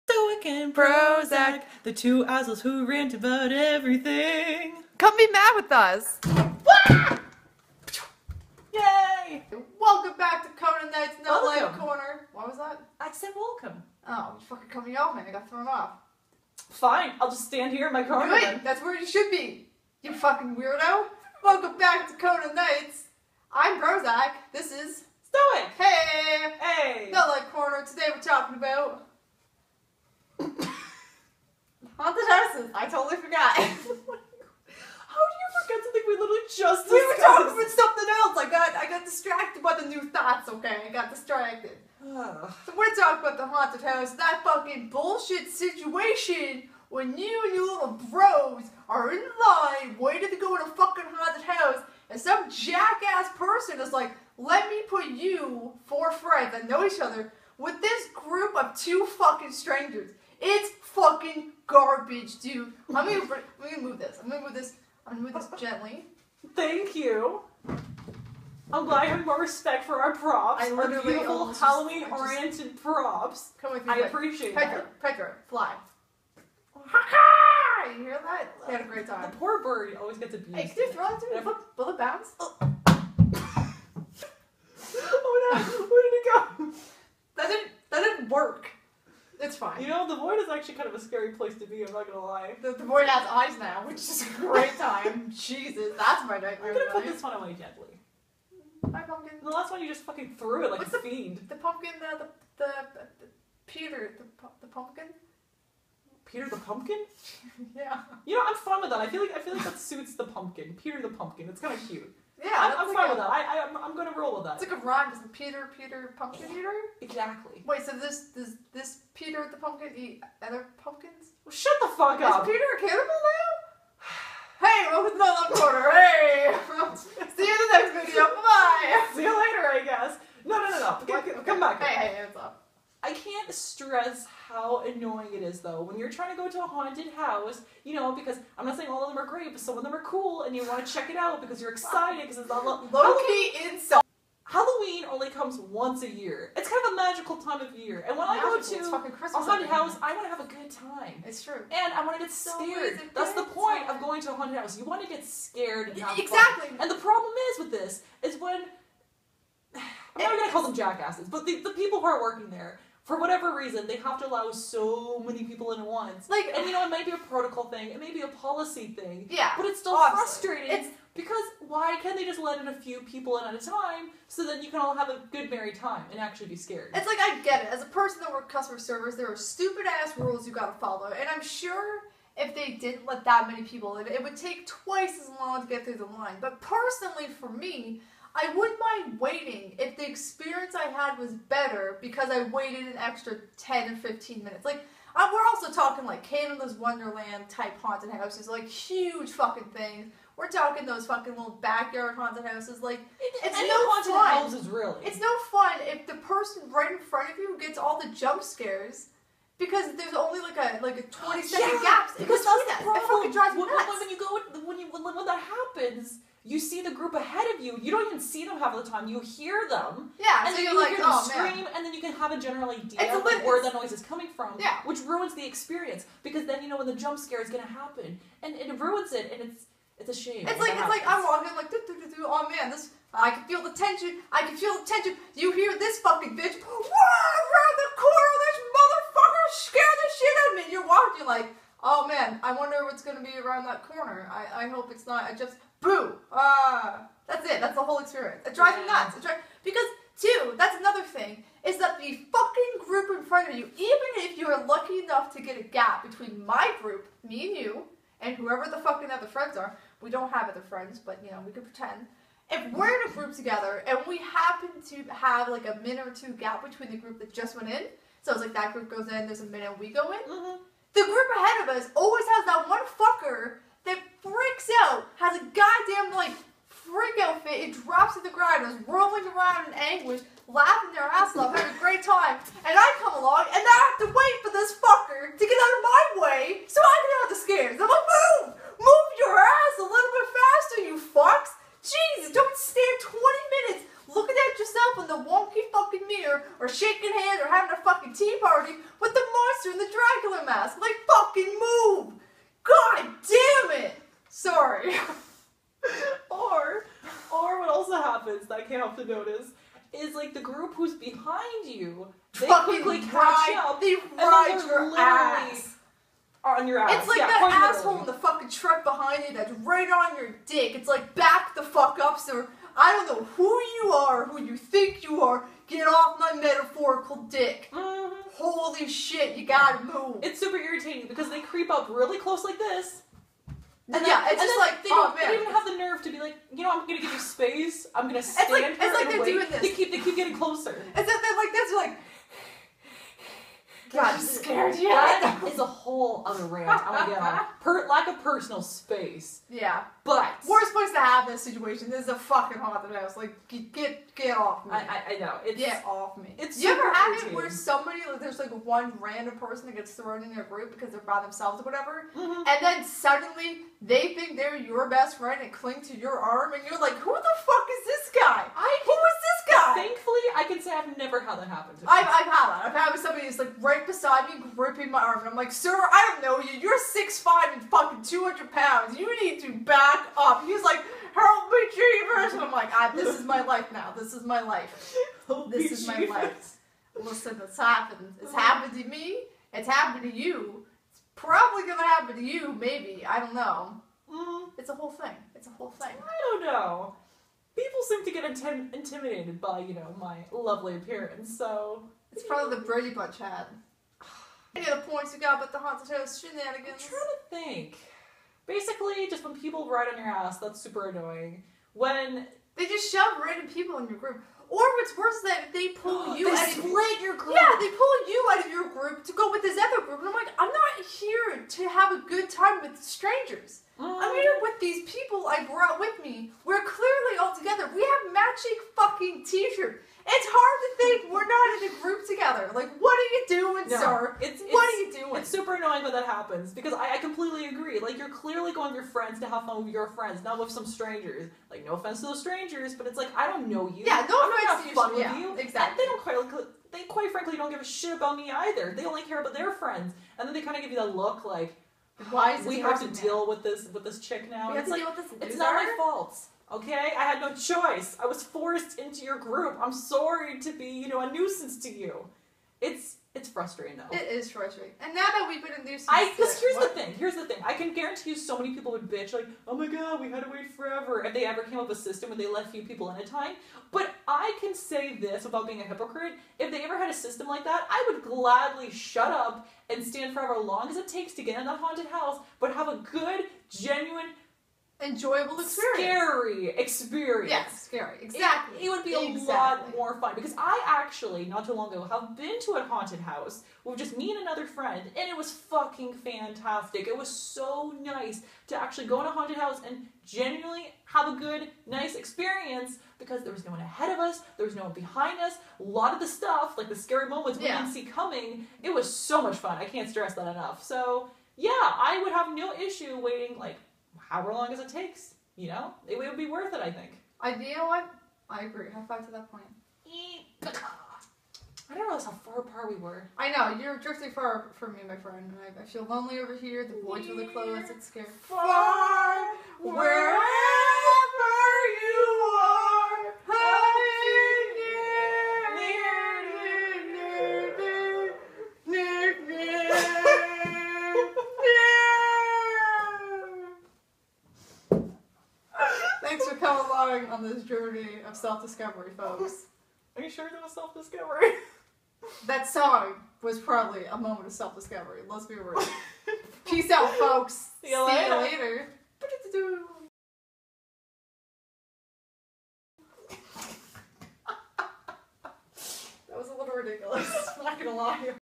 HAUNTED HOUSES, GET ORGANIZED!
Welcome back to the No Life Corner with Stoic and Prozac. In today's episode, we discuss how much of a pain in the ass haunted house's queues are. Relatable every Halloween, no joke.